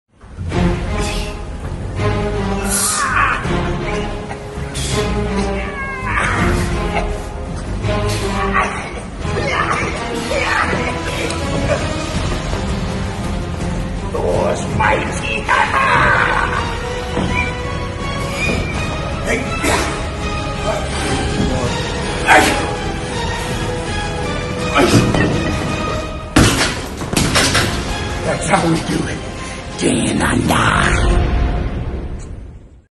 Jack Black si Allena come Thor - Chris Hemsworth
JackBlack prova ad allenarsi come ChrisHemsworth nel suo difficilissimo workout da

CineMagicFilmIsNow è il punto di riferimento per gli appassionati di cinema, serie TV, Netflix 😍